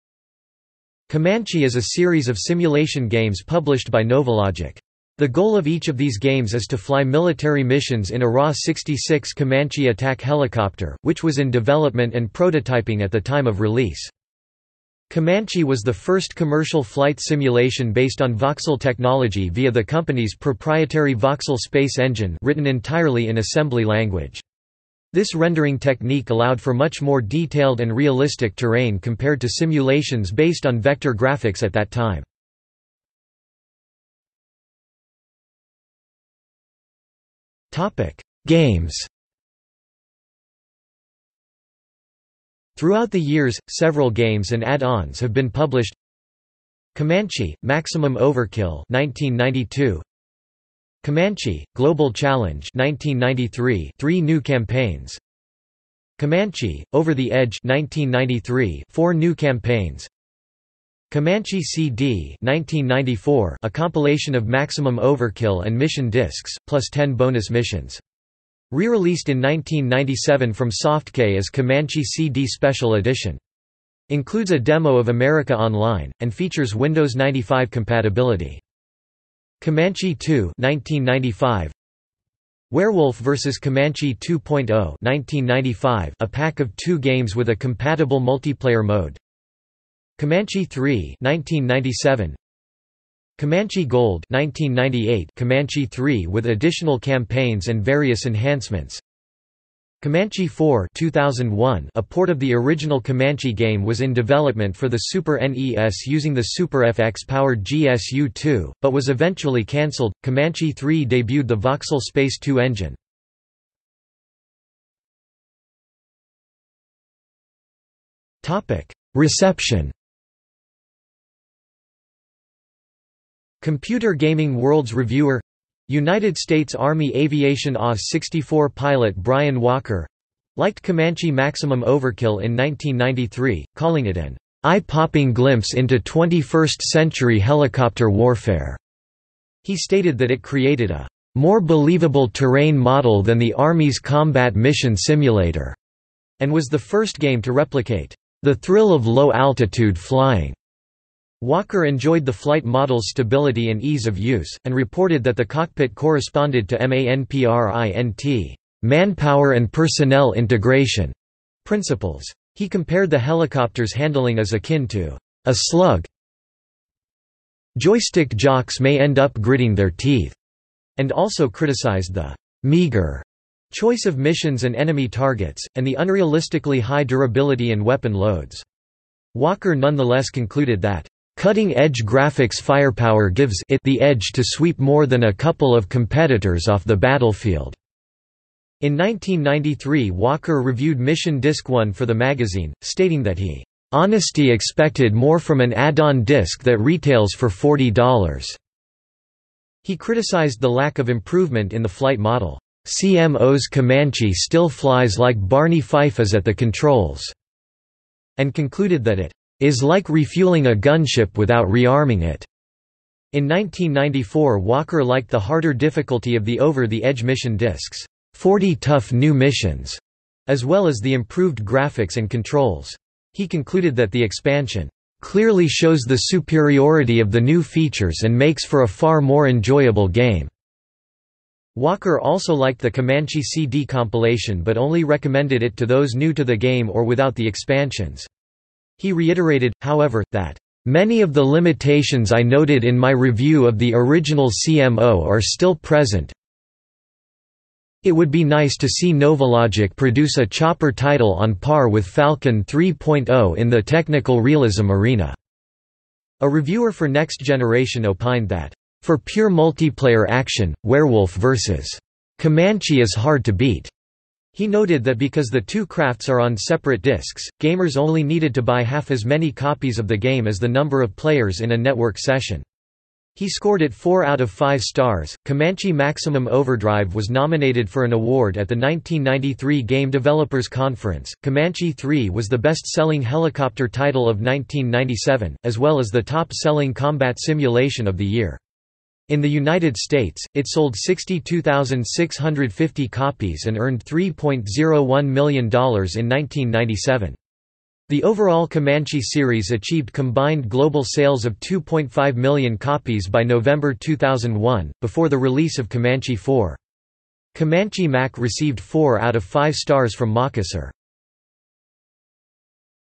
Comanche (video game series) | Wikipedia audio article
00:00:34 1 Games
00:01:54 2 Reception

- Socrates

SUMMARY
Comanche is a series of simulation games published by NovaLogic. The goal of each of these games is to fly military missions in a RAH-66 Comanche attack helicopter, which was in development and prototyping at the time of release.
Comanche was the first commercial flight simulation based on voxel technology via the company's proprietary Voxel Space engine (written entirely in assembly language). This rendering technique allowed for much more detailed and realistic terrain compared to simulations based on vector graphics at that time.